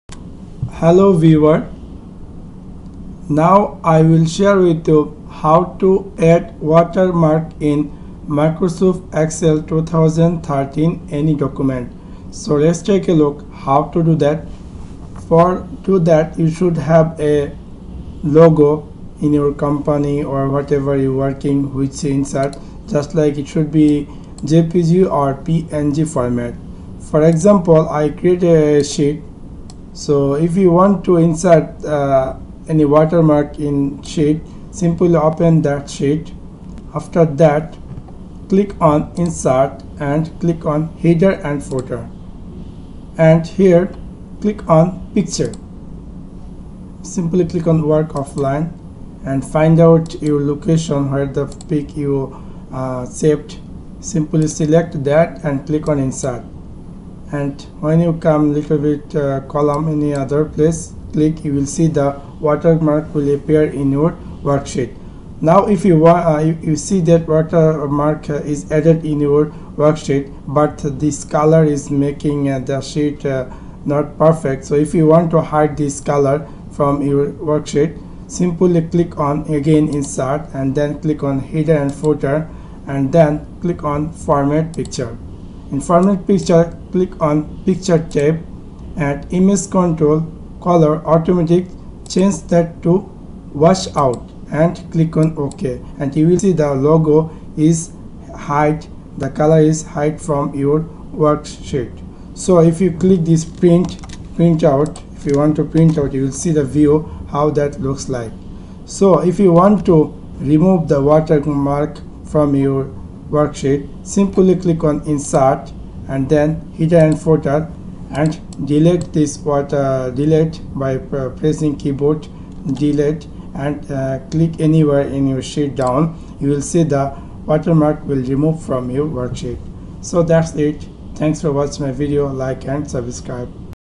How To Add A Watermark To A Worksheet In Microsoft Excel 2013
In this tutorial you will learn how to add a watermark to a worksheet in Microsoft Excel 2013.

Adding a watermark to your Excel worksheet makes it look cool and it may come handy if you want to enhance your company documents, say, the lists of workshop participants that you want to print out and even your company invoices!

Adding a watermark to a worksheet in Microsoft Excel 2013 does not happen just in a couple of clicks, but is still quite easy to do. Before you proceed, make sure you have your image file prepared. A nice JPG or PNG image with your logo or any other watermark-suitable picture is okay.

Step 1. Open your ready worksheet in Excel.

Step 2. From the menu ribbon on top of the file, click "Insert" tab.

Step 3. From the tab's "Text" section, pick "Header & Footer".

Step 4. The "Header & Footer Tools Design" tab will display. Click on the "Picture" button to add a picture. In the new dialog box, choose the way to add an image file. Navigate to the location of your image file, select it, and click "Insert".

Step 5. Now you'll see your picture is represented by "&[Picture]" in your Excel table. Depending in the size of your picture file, you can insert blank lines before "&[Picture]" to center the image on the worksheet.

Click on any table cell outside of the header. The image will display behind the text and graphics.

Step 6. Open the header again to invoke the "Header & Footer Tools Design" tab. Choose "Format Picture" from the "Header and Footer Elements" section.

Step 7. In the newly appearing "Format Picture" dialog box, choose the "Picture" tab. From the drop-down menu of "Color", select: "Washout". Click "Ok". You can see now your faded picture behind your table! Now we can print out our Excel table together with a watermark.

Step 8. If you want to remove the watermark, open the header, select the picture or the text marker (&[Picture]), delete it, and click on any cell outside the header.

Result: Congratulations! You've just learned how to add a watermark to a worksheet in Microsoft Excel 2013.